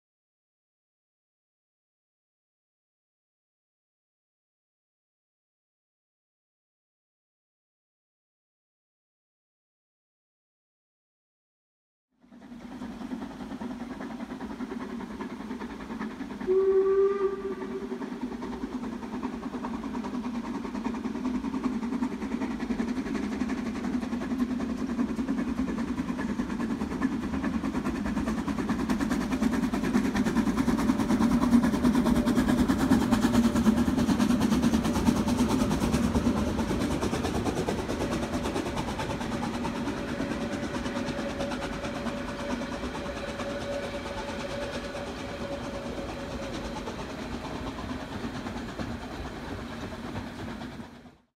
Harzer Schmalspurbahn am Brocken - Harz narrow-gauge railway climbing Brocken (steam locomotive)
Steam locomotive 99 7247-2 of the Harz narrow-gauge railway climbing Brocken (1,141m) in the Harz mountains, Germany.
Recorded June 2018 on iPhone 6S.

Locomotive data:
Construction year: 1956
Manufacturer: Lokomotivbau Karl Marx Babelsberg (LKM)
Max. speed: 40km/h
Total weight: 61,0t
Coal reserve: 4,0t
Water reserve: 8,0t

Dampflokomotive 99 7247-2 der Harzer Schmalspurbahn erklettert den Brocken (1,141m) im Harz.
Im Juni 2018 mit dem iPhone 6S aufgenommen.

Daten zur Lokomotive:
Herstellungsjahr: 1956
Hersteller: Lokomotivbau Karl Marx Babelsberg (LKM)
Max. Geschwindigkeit: 40 km/h
Gesamtmasse: 61,0t
Kohlevorrat: 4,0t
Wasservorrat: 8,0t